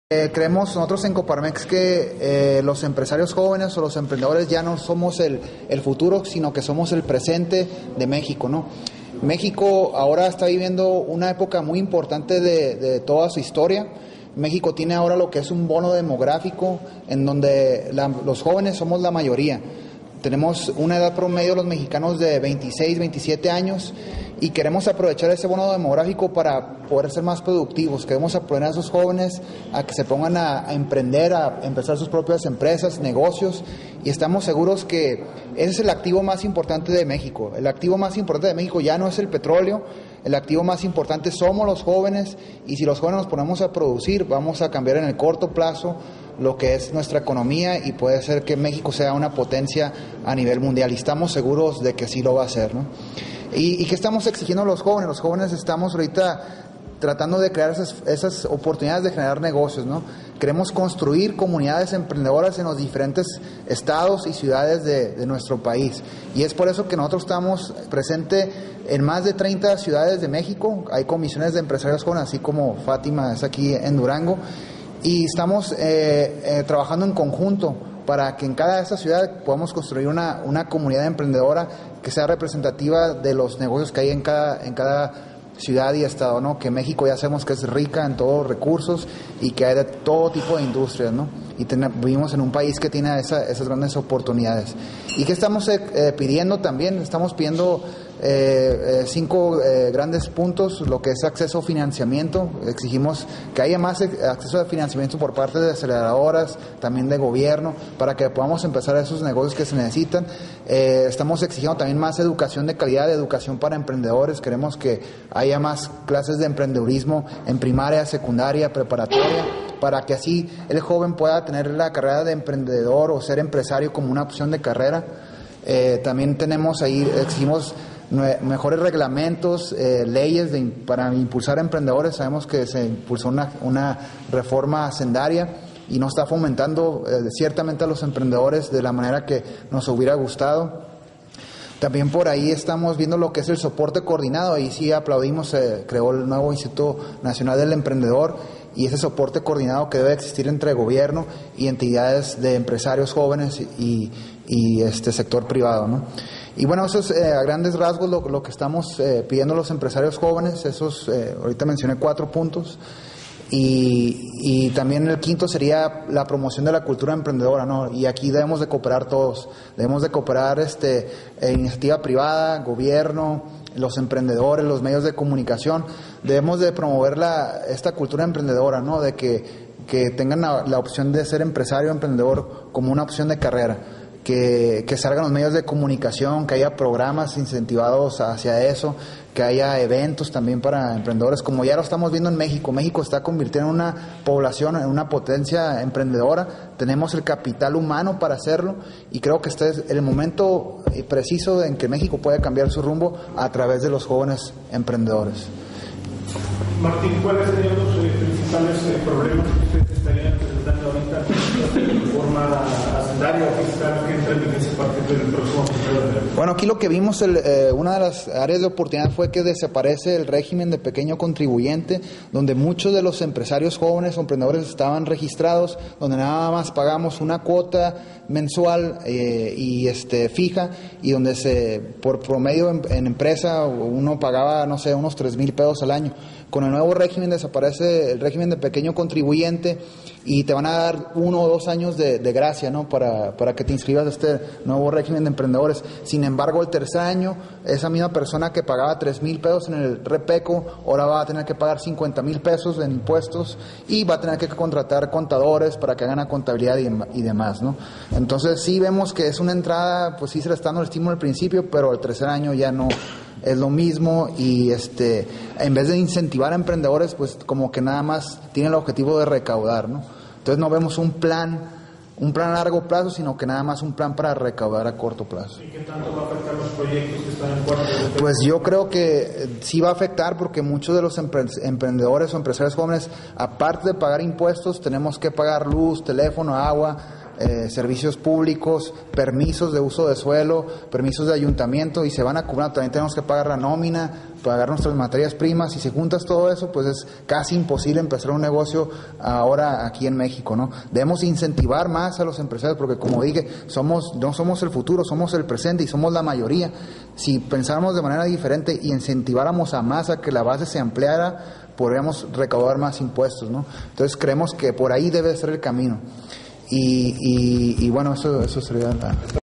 Rueda de Prensa COPARMEX,Con el Presidente Nacional de la Comisión de Empresarios Jóvenes Martín Rom
Comenta sobre los proyectos importantes que le hace falta al país para el crecimiento de empresas debido  a la aprobación de la reforma hacendaría, así mismo menciona 5 puntos importantes que no contiene está, sobre todo incorporar prestamos a jóvenes empresarios para el crecimiento del país ya que los jóvenes son el presente de México.